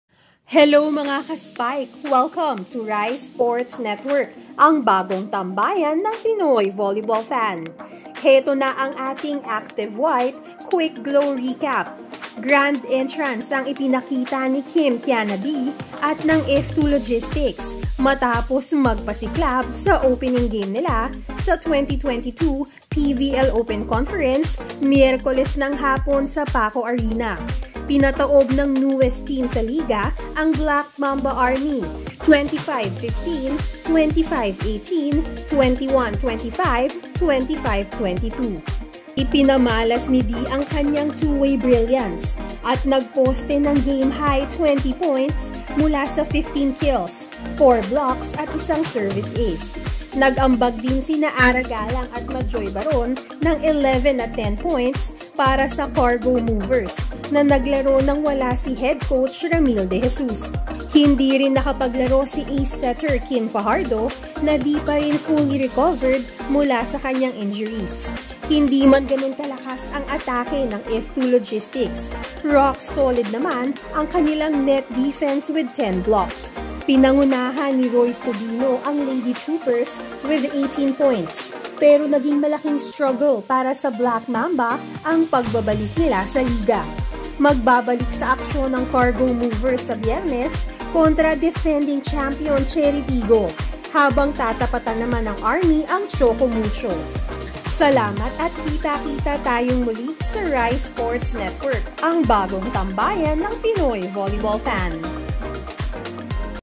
WELCOME BACK: KKD, nagpakitang gilas sa big win ng F2 Logistics
Pinangunahan ni Kim Kianna Dy ang atake ng F2 Logistics upang lunurin ang Black Mamba Army sa kanilang matchup.